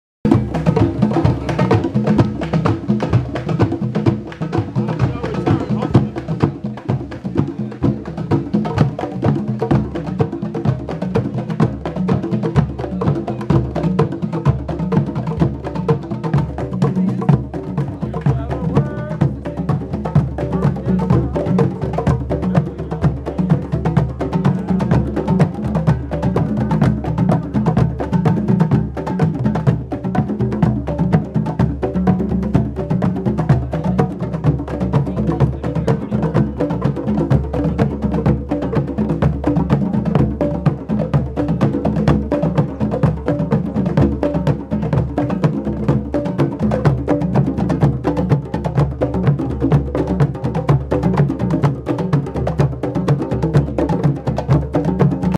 Sunday, 12 October 2014 DRUM CIRCLE 4/30 @ Meridian Hill Park / Washington DC
Video clip 4/30 of the MALCOLM X. PARK DRUM & DANCE CIRCLE at Meridian Hill Park / Upper Level between 15th and 16th Street and W and Euclid Street, NW, Washington DC on Sunday evening, 12 October 2014 by Elvert Barnes Photography